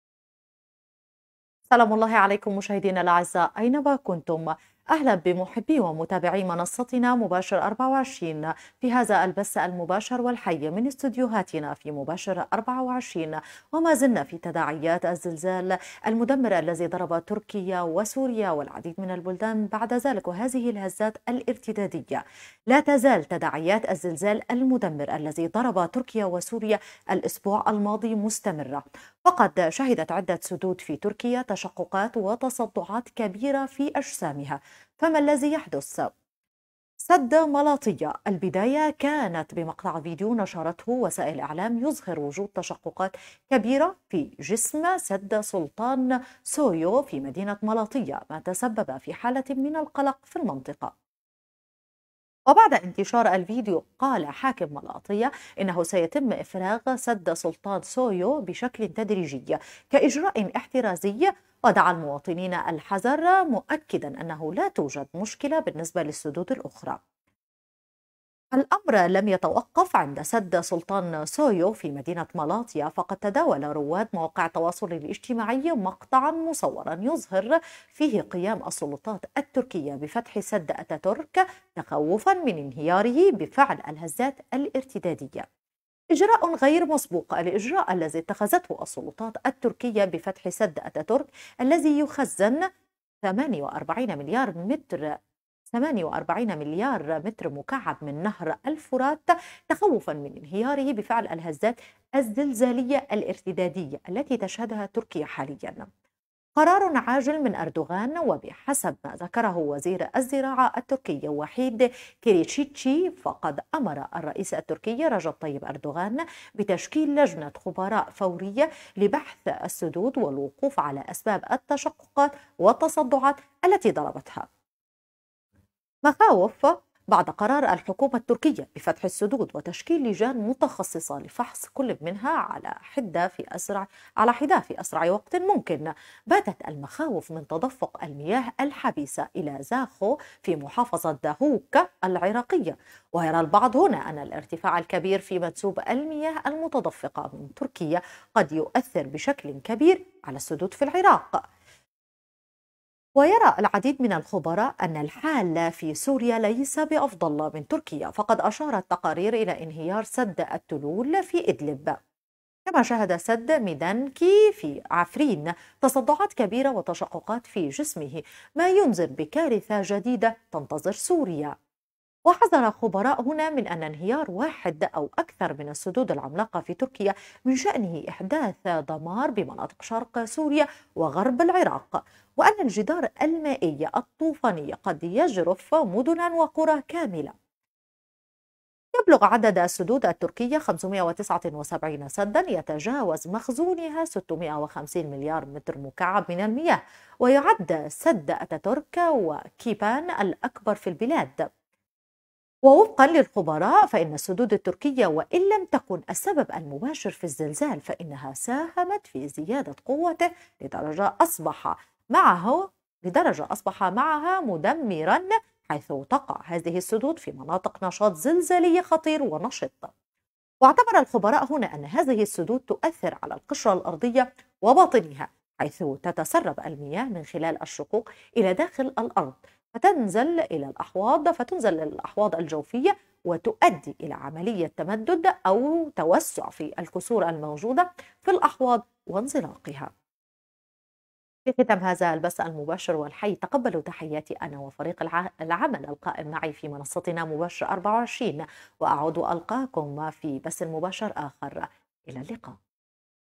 بعد ظهور تشققات في السدود التركية.. قرار عاجل من أردوغان.. وهذا هو موقف السدود في سوريا والعراق
لا تزال تداعيات الزلزال المدمر الذي ضرب تركيا وسوريا الأسبوع الماضي مستمرة، فقد شهدت عدة سدود في تركيا تشققات وتصدعات كبيرة في أجسامها.. فما الذي يحدث؟